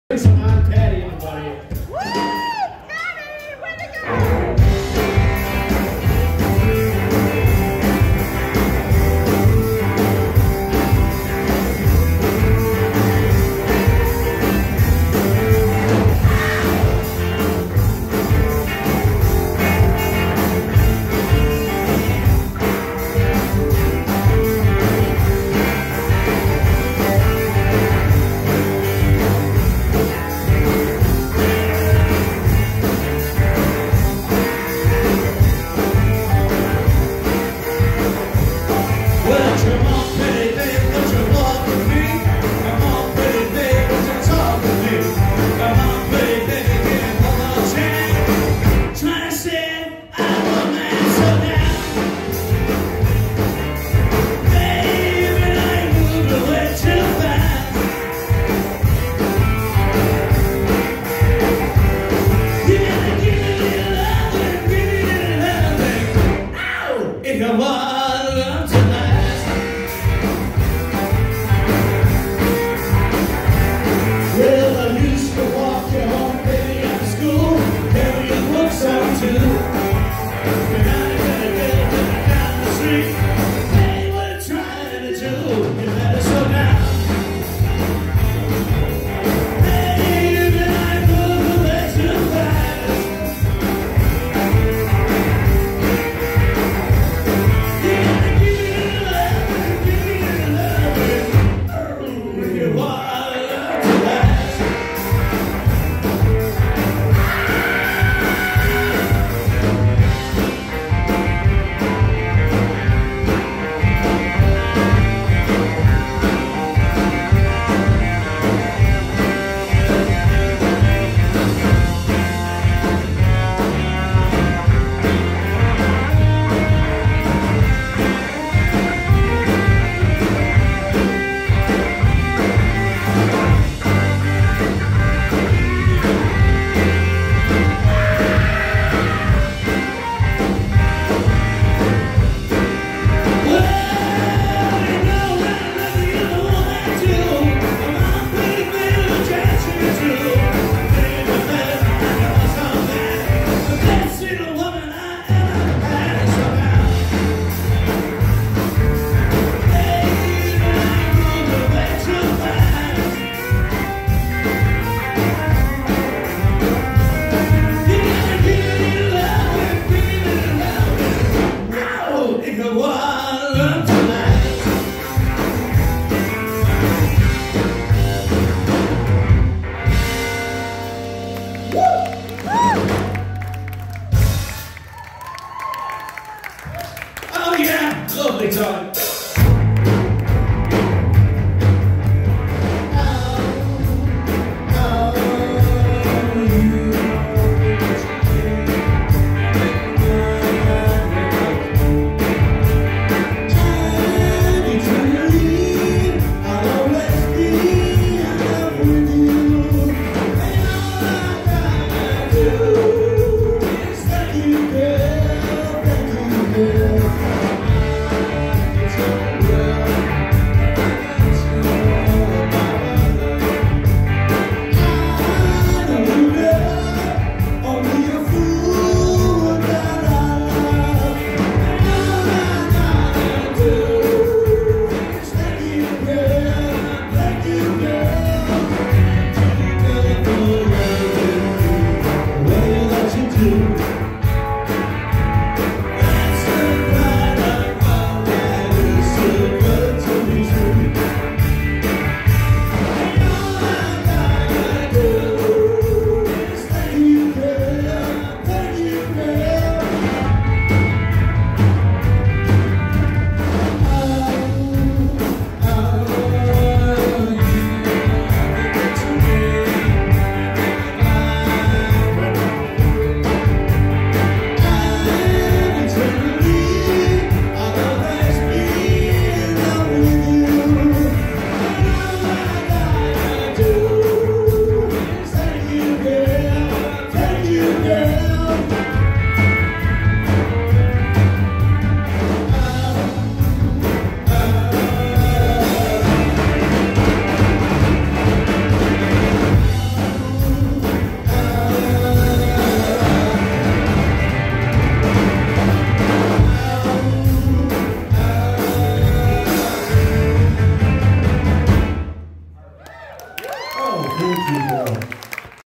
Tom's Beatle Band 2
Lennon Remembered at the Beachland Ballroom October 9, 2019